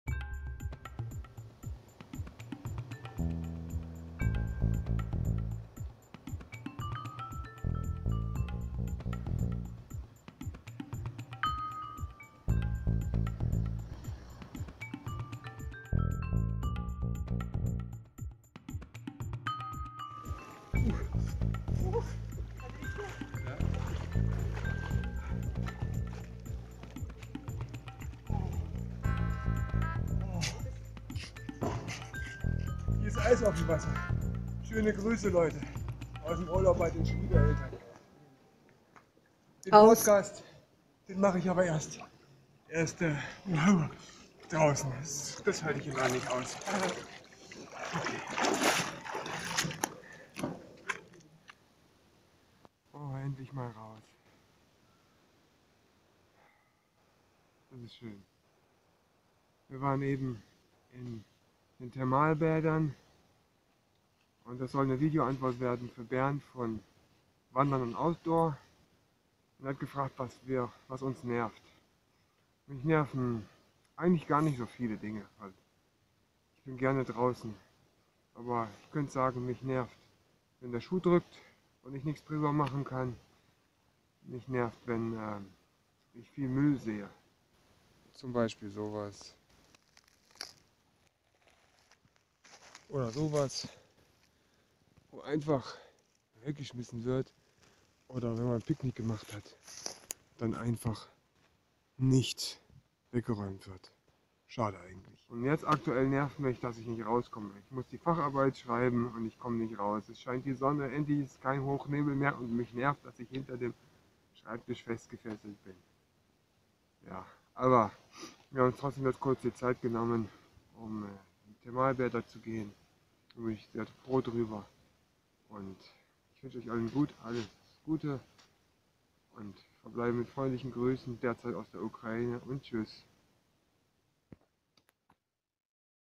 Was nervt draußen? [VA für Bernd von Wandern & Outdoor]
Eigentlich nervt mich draußen nichts so schnell. Oder ... ?
Nur drinnen kapituliere ich an der Semesterarbeit, während draußen nach wochenlangem Hochnebel endlich wieder die Sonne hervorkommt.

LG aus der Ukraine nach 2016. Möge es gesegnet Jahr für Euch alle sein.
benjamin:)